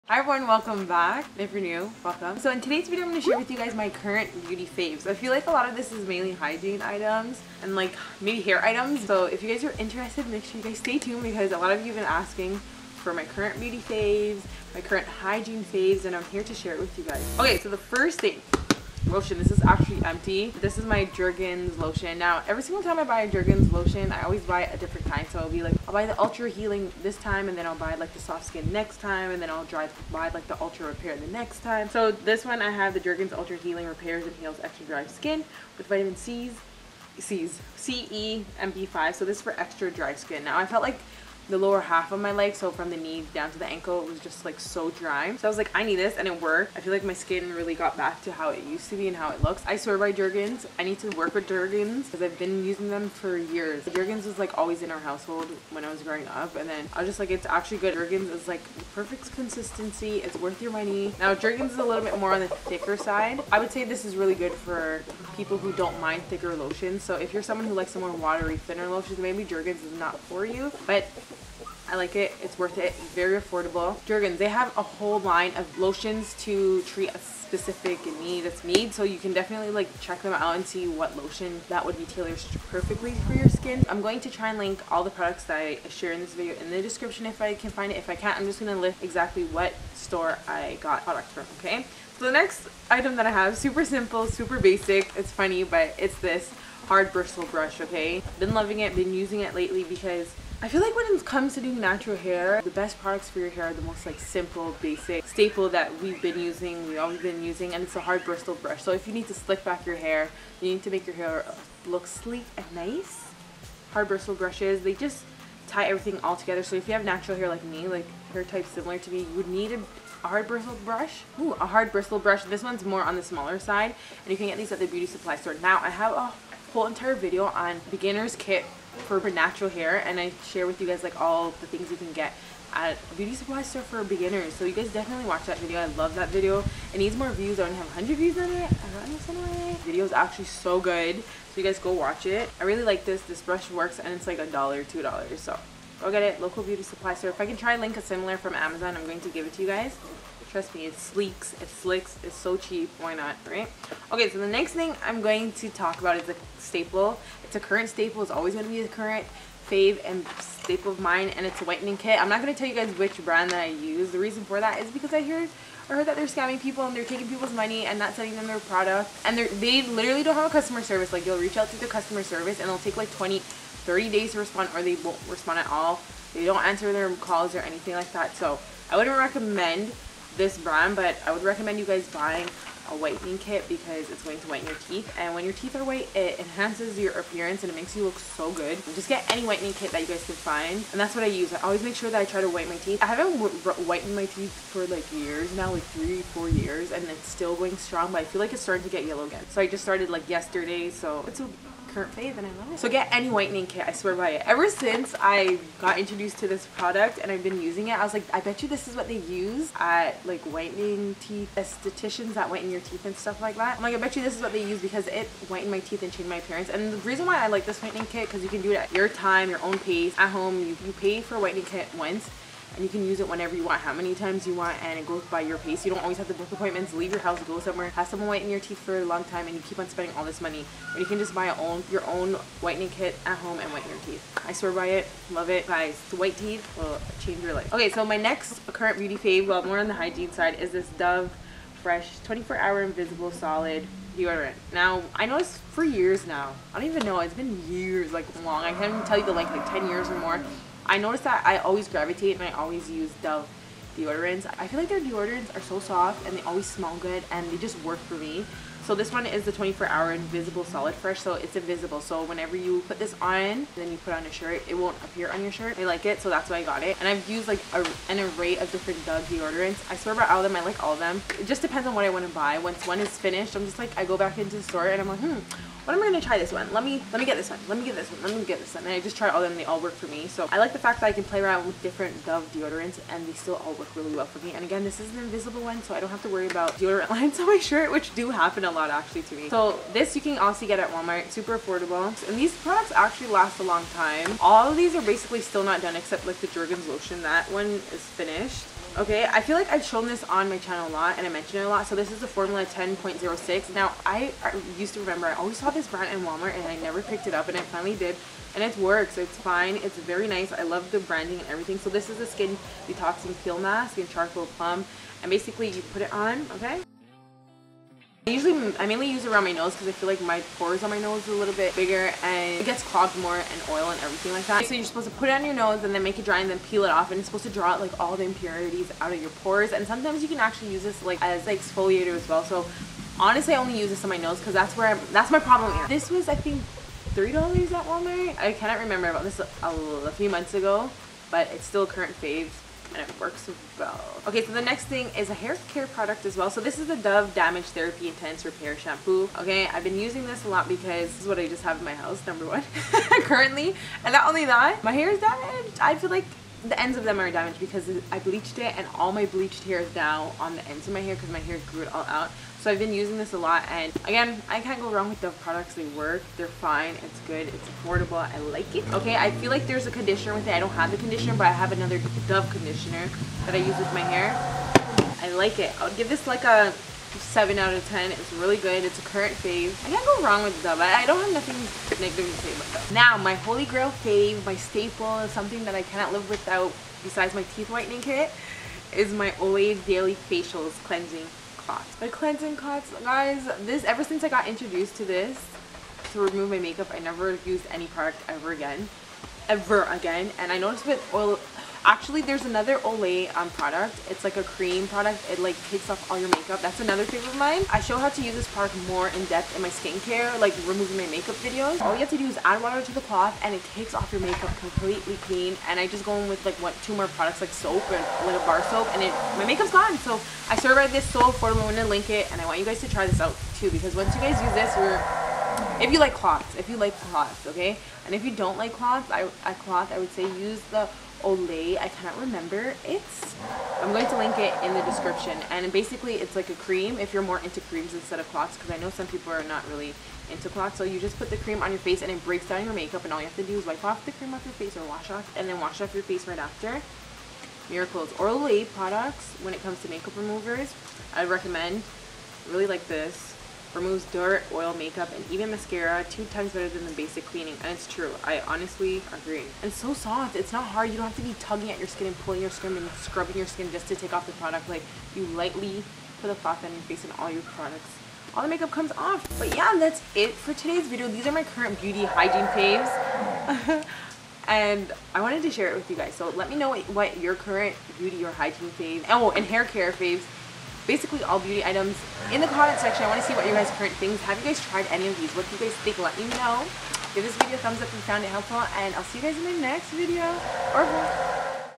DRUGSTORE BEAUTY FAVS I CANT STOP USING 😍+ hygiene and haircare | Annesha Adams
My affordable feminine beauty, natural hair care, and hygiene favorite products/empties, that make me look and feel better. Amazing products you need at all affordable prices.

SHOP MENTIONED PRODUCTS:

Jergens Ultra healing lotion - walmart

Hard bristle brush - beauty supply store

Here's quick access to all of my favorite tech + beauty items